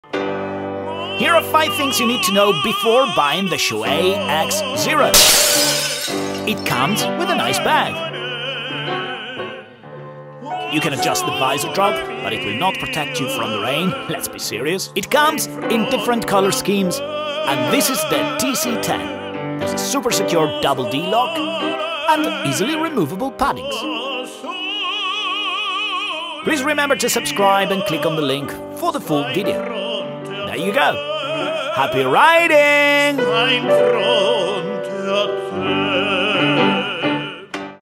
5 things to know before you buy a Shoei Ex-Zero
Here is a sneaky look at my video unboxing and testing the Shoei-ExZero Helmet check the full video